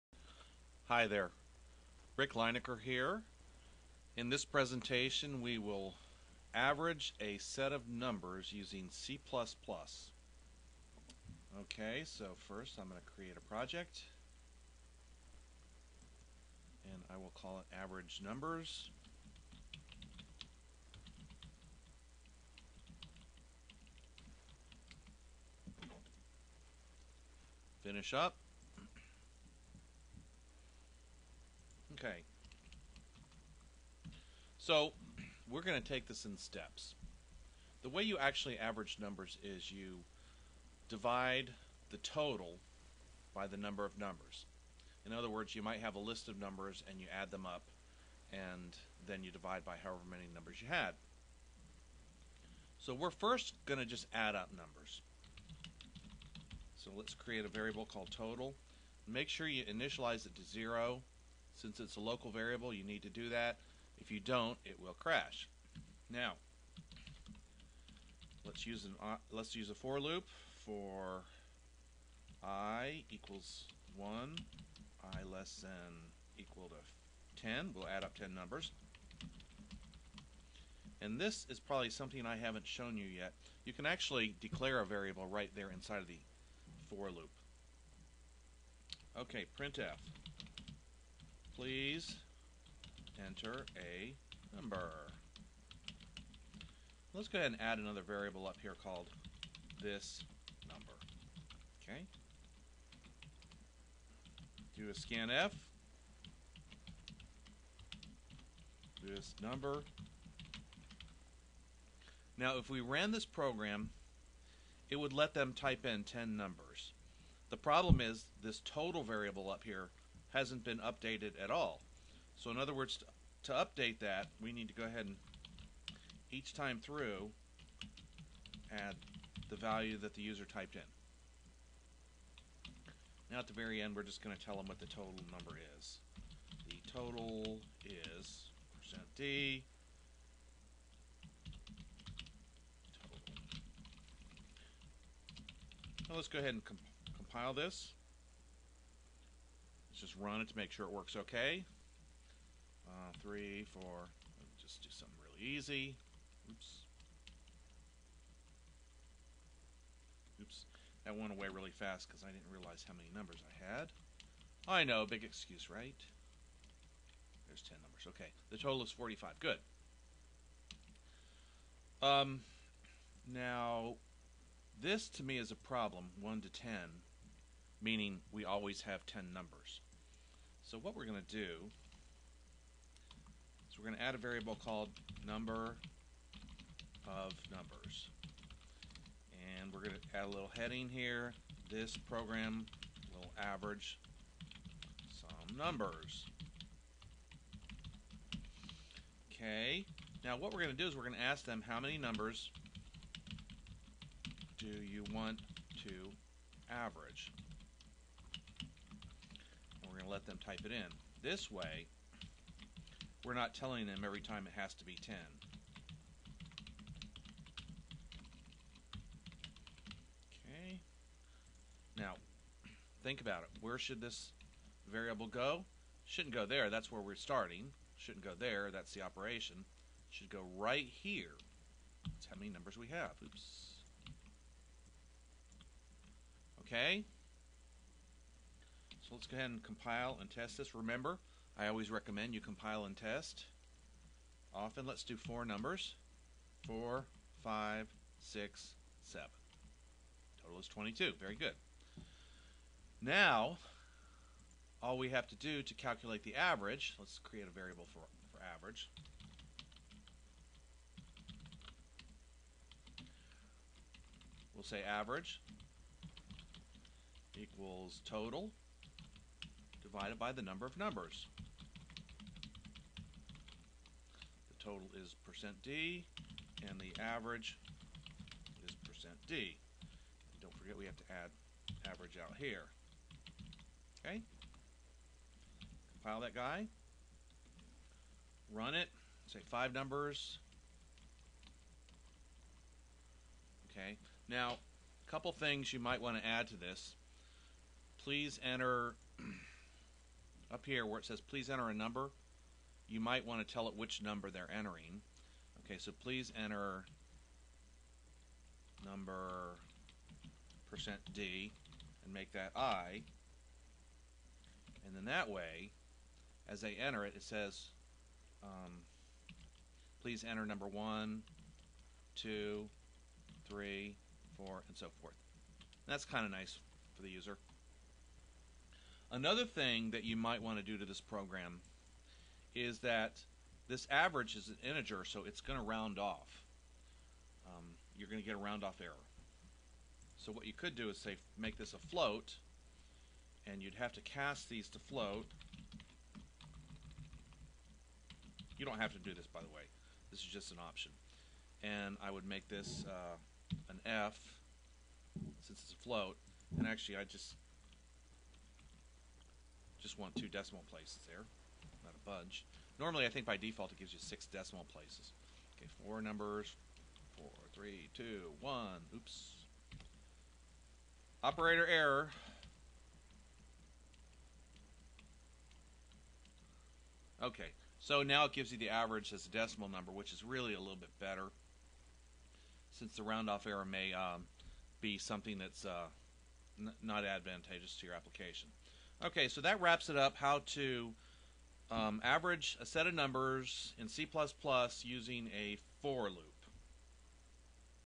Averaging Numbers with C++
This presentation shows you how to average numbers using C++.